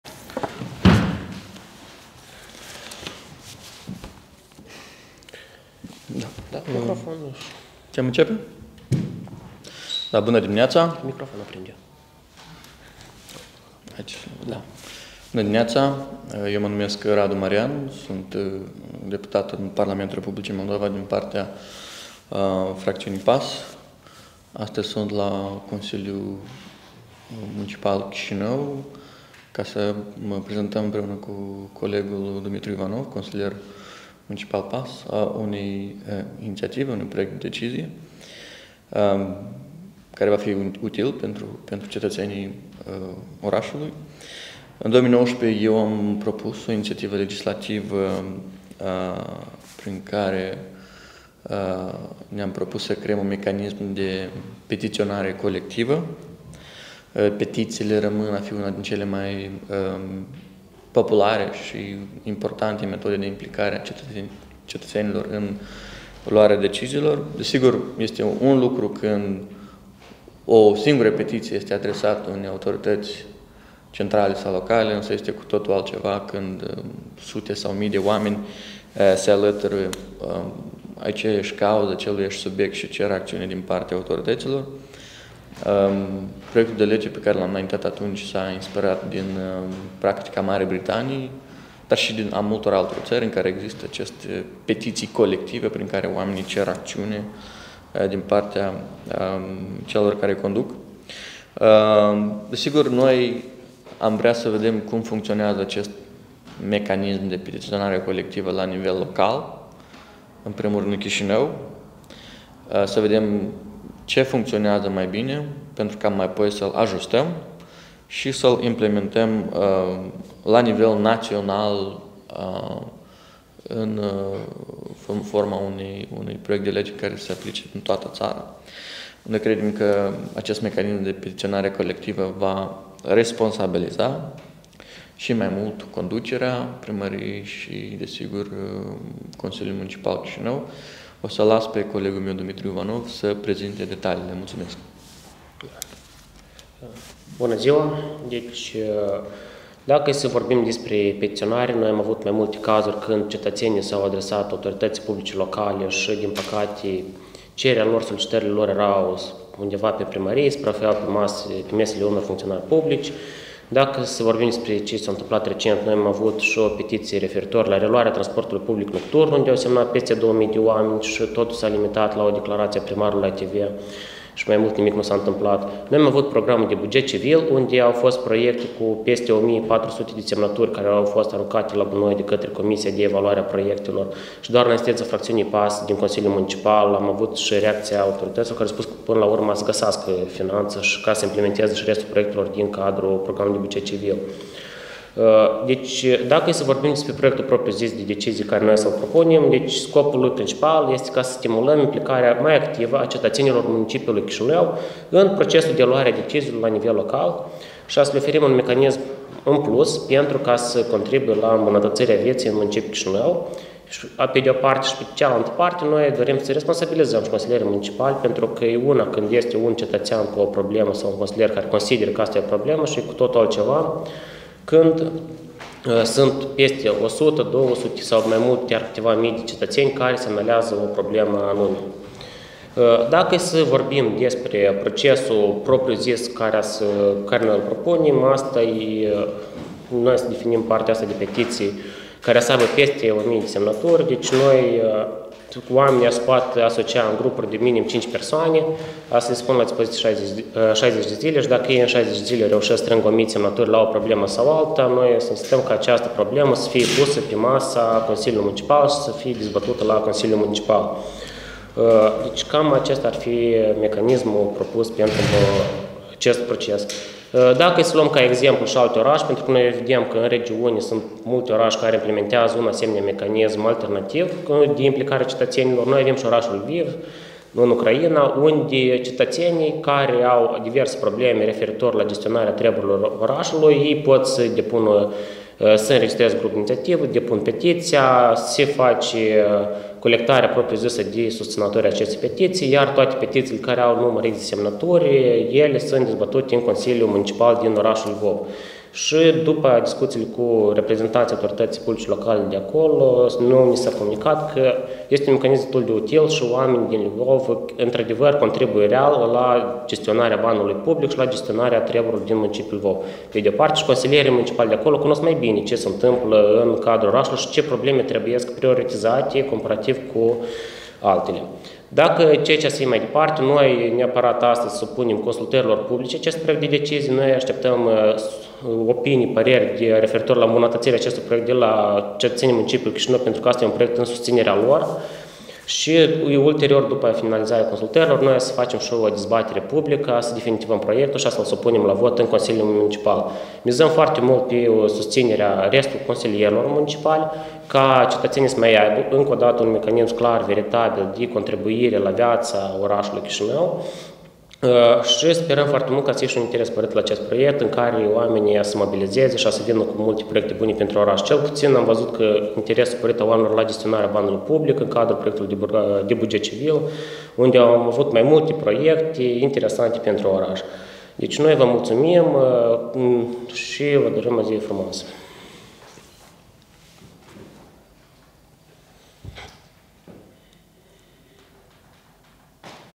Conferință susținută de deputatul PAS, Radu Marian și consilierul municipal PAS, Dumitru Ivanov
Conferință de presă susținută de către deputatul Partidului Acțiune și Solidaritate, Radu Marian, și consilierul municipal PAS, Dumitru Ivanov